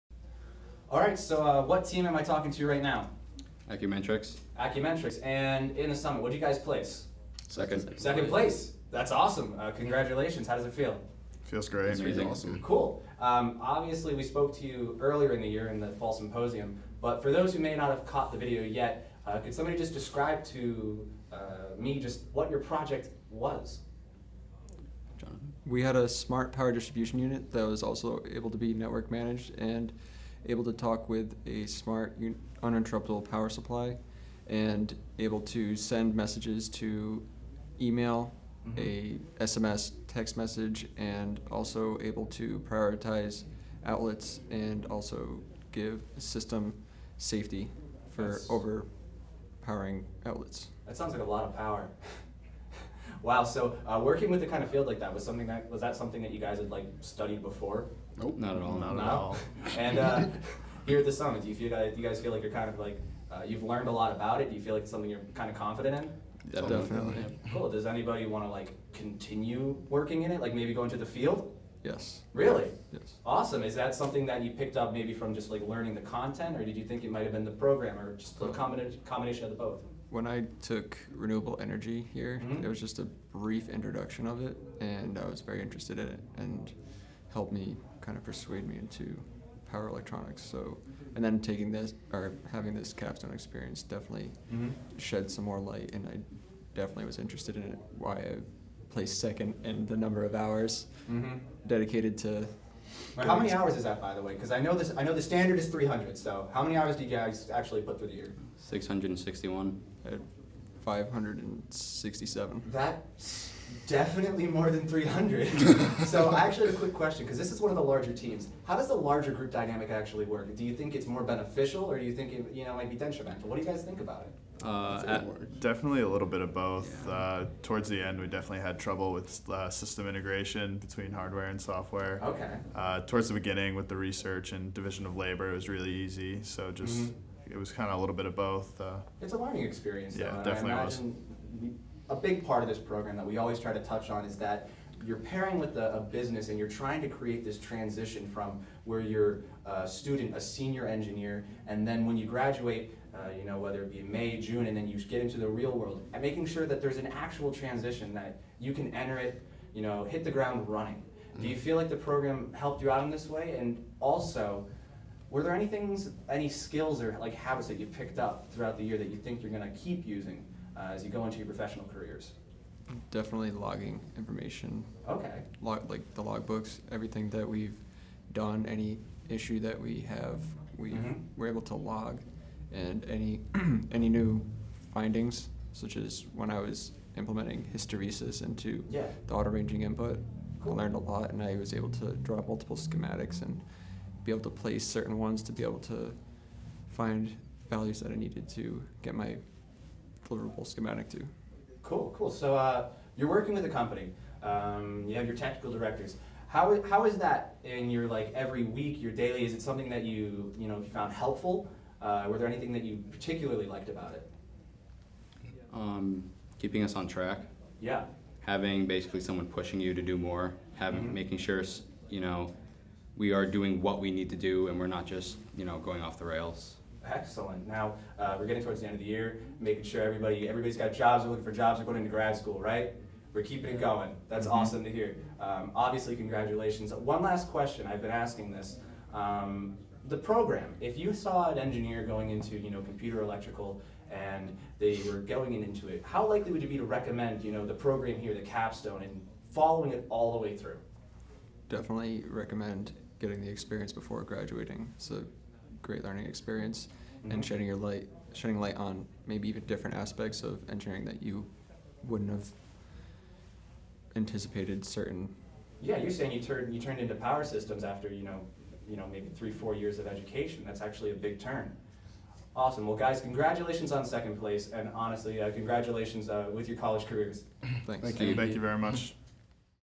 ELECOMP Interviews: Team Acumentrics, 2018 ELECOMP Capstone Summit
Team Acumentrics discusses their ELECOMP Capstone Design Project and the ELECOMP program available at the University of Rhode Island.